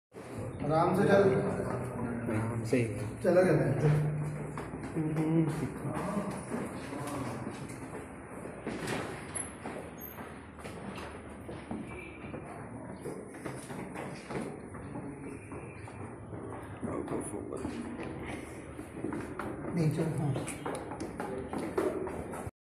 Honor 8C 1080p 30fps video recording sample
Recorded using Honor 8C at the Beebom office